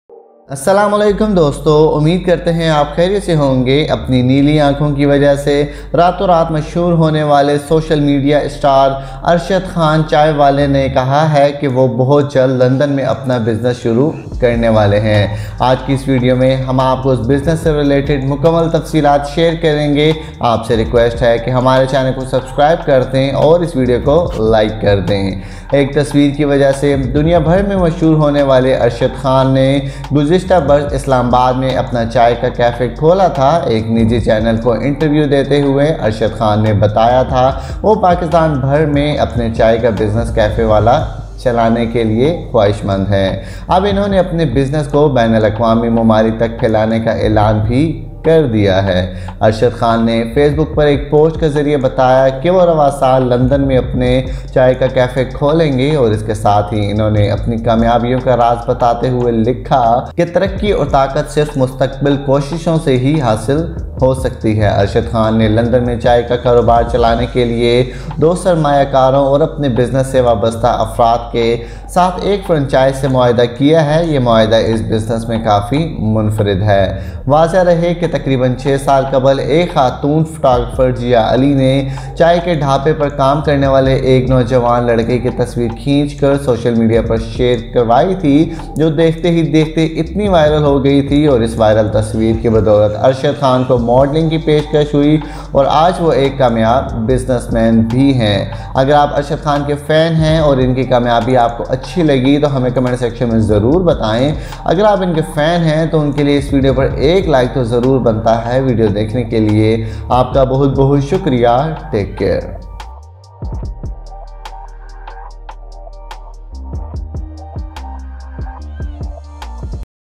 Arshad Khan Chaiwala set to open chain of cafes in London | Viral Chaiwala | Cafe Chaiwala
Arshad Khan Chaiwala set to open chain of cafes in London |  Viral Chaiwala